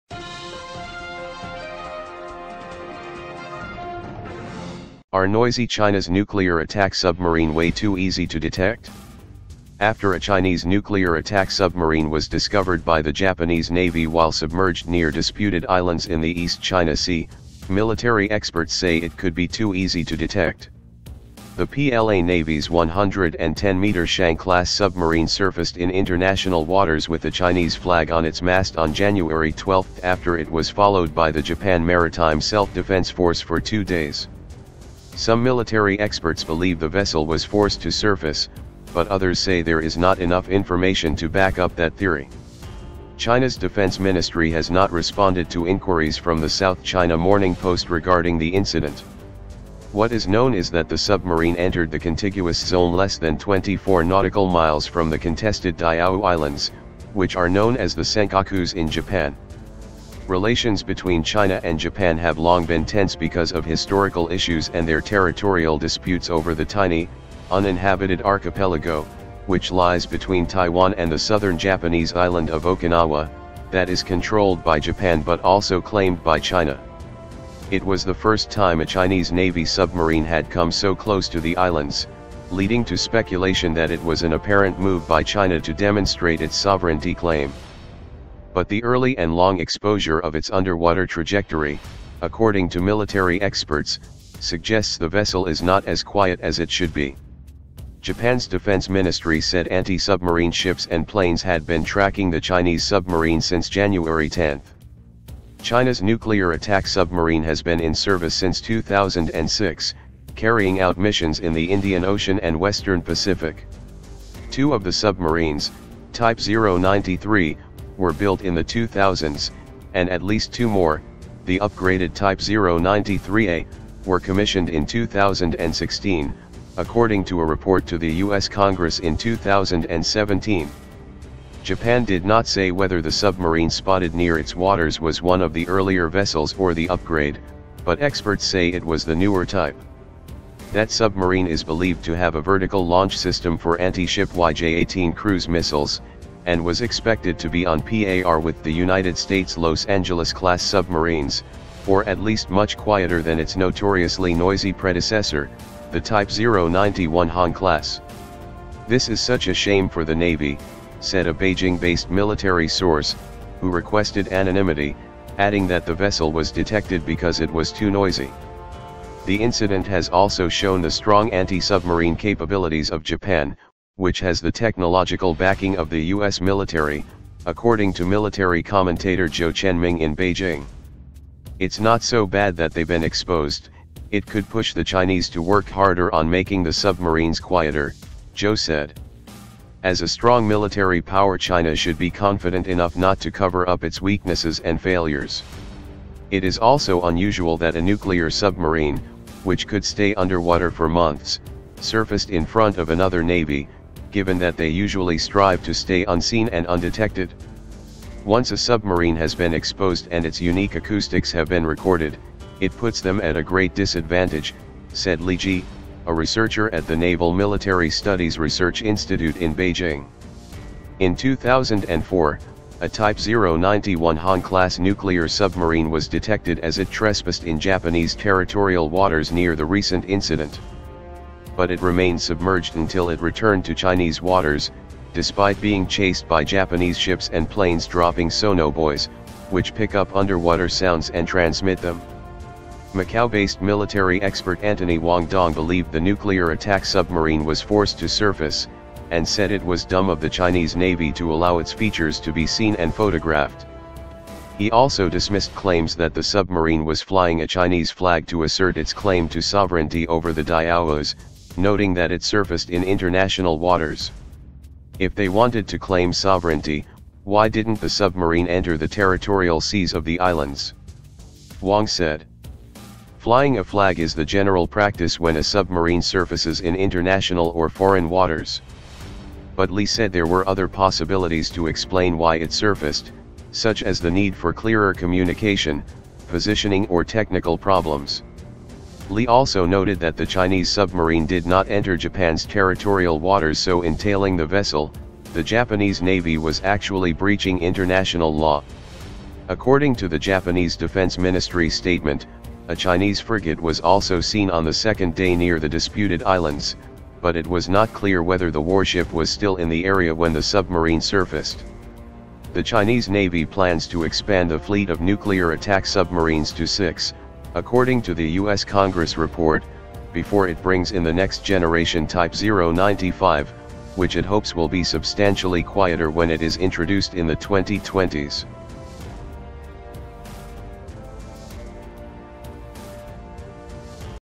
Are Noisy China’s nuclear attack submarine way too easy to detect?
After a Chinese nuclear attack submarine was discovered by the Japanese navy while submerged near disputed islands in the East China Sea, military experts say it could be too easy to detect.

The PLA Navy’s 110-metre Shang-class submarine surfaced in international waters with a Chinese flag on its mast on January 12 after it was followed by the Japan Maritime Self-Defence Force for two days.